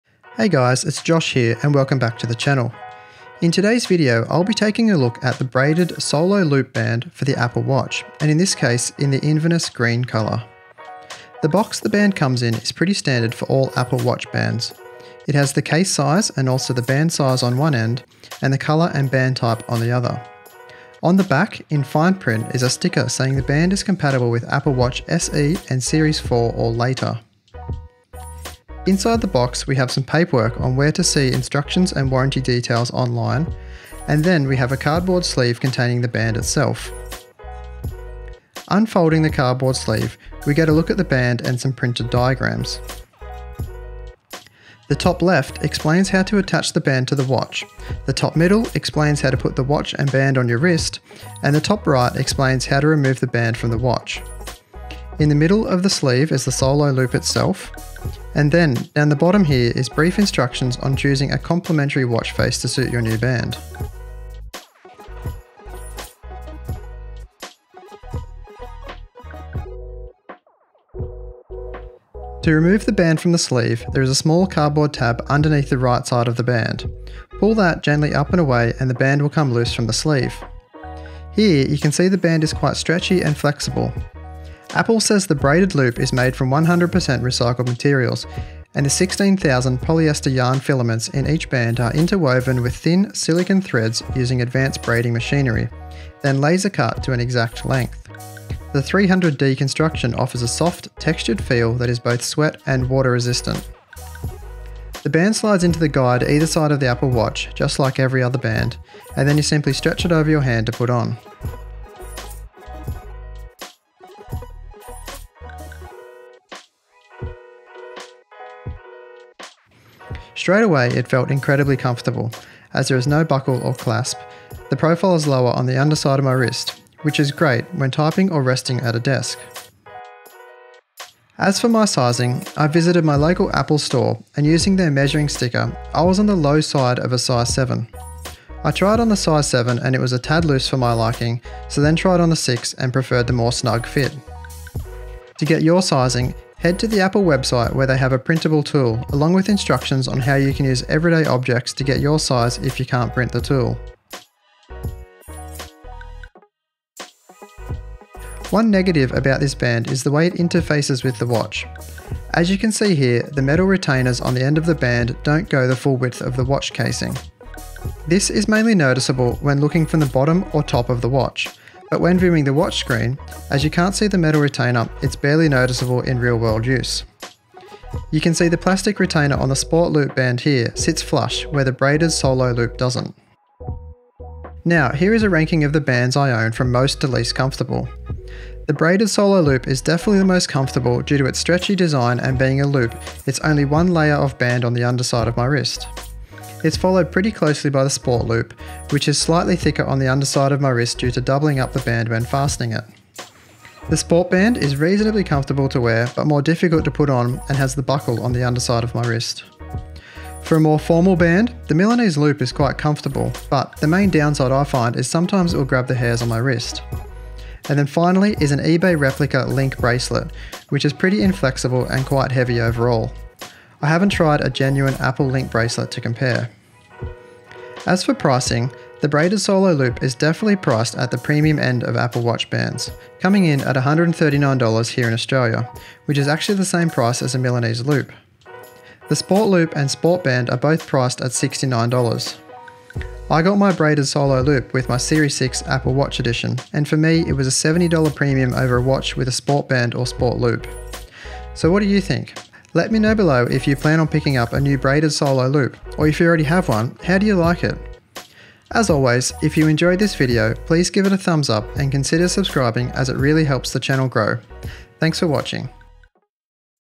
Apple Watch Braided Loop Review
In this review video I take a look at the Apple Watch Braided Loop.

I recently got a new Apple Watch Edition and my selected band was the braided solo loop in Inverness Green. After trying on a couple of sizes at the Apple Store, I settled on a size 6, and it would have to be the most comfortable watch band I've tried.

Watch the video to see an unboxing, some close ups, a downside and some comparisons.

Camera gear I use to film videos